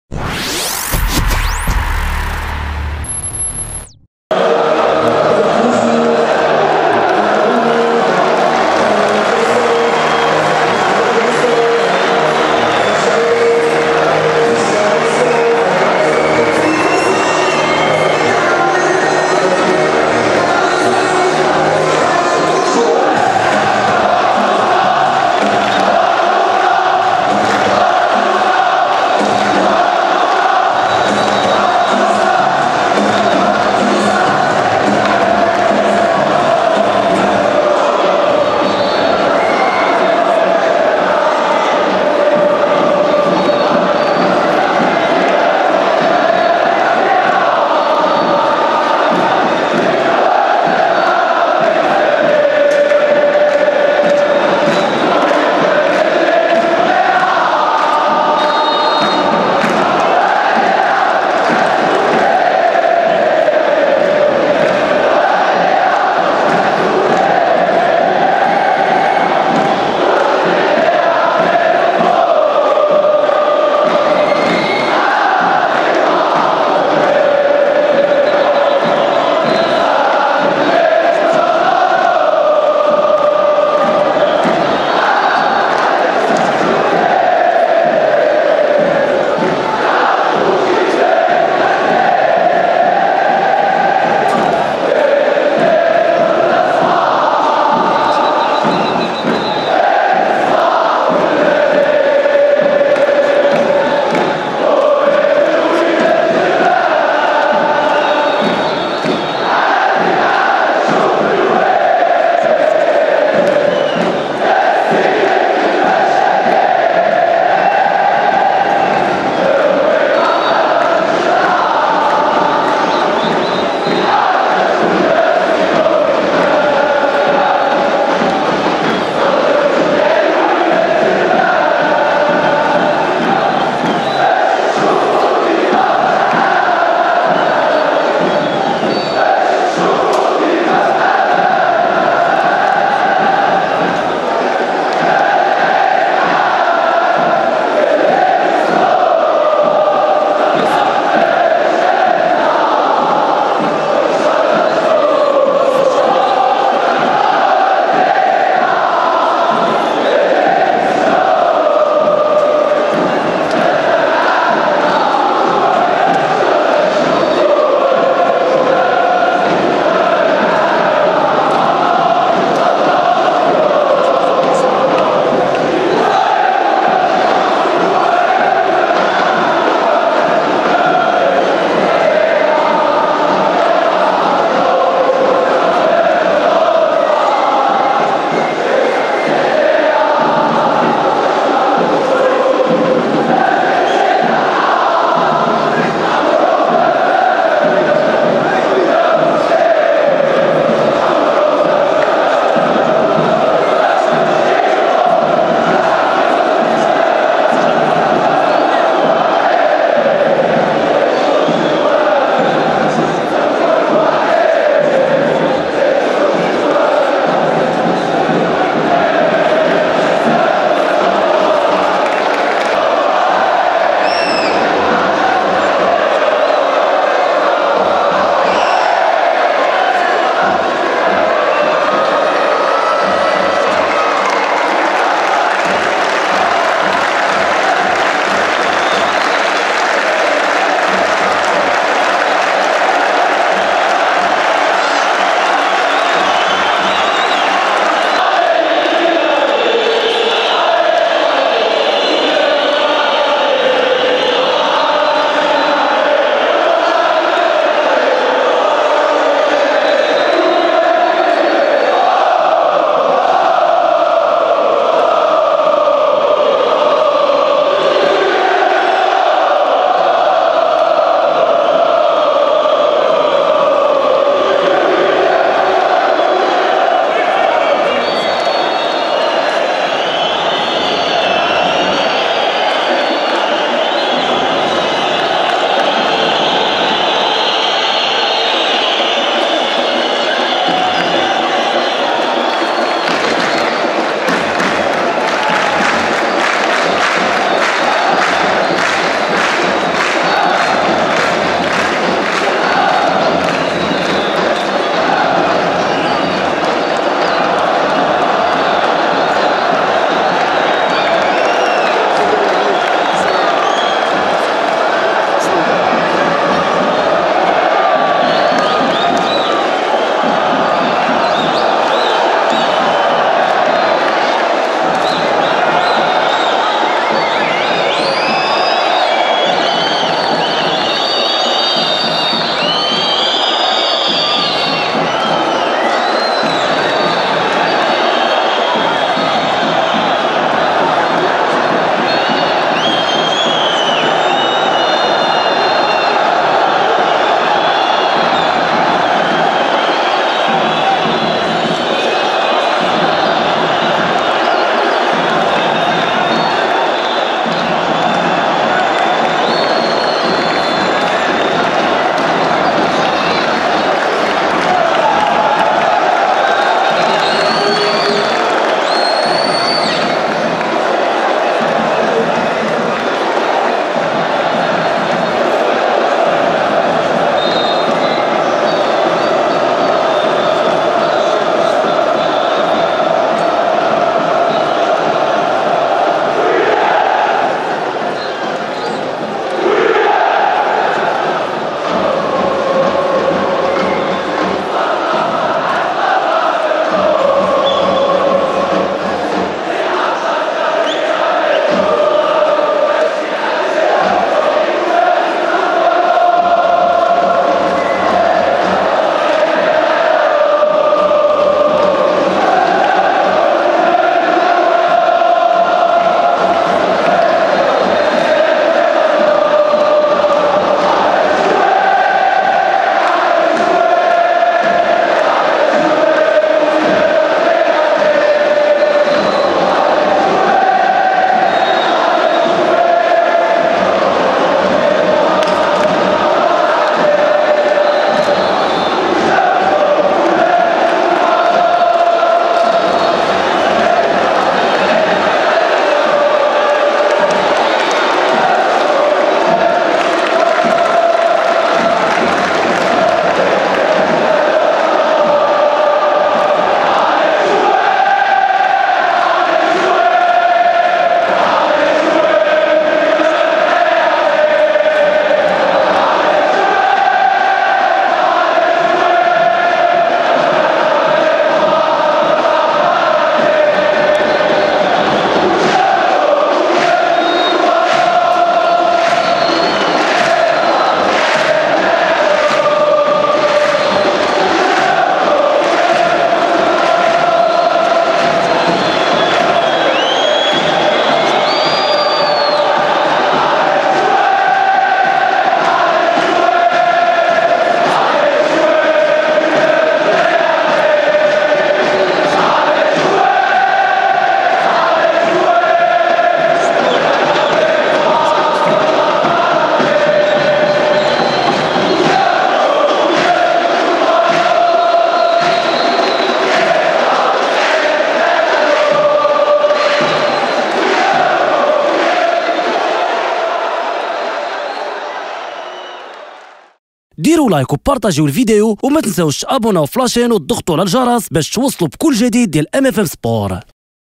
الوينرز تحتفل بعيدها 14 ب 2 تيفوات رهيبة.. 9 دقائق من الإمتاع و الإبداع
الوينرز تحتفل بعيدها 14 ب 2 تيفوات رهيبة   9 دقائق من الامتاع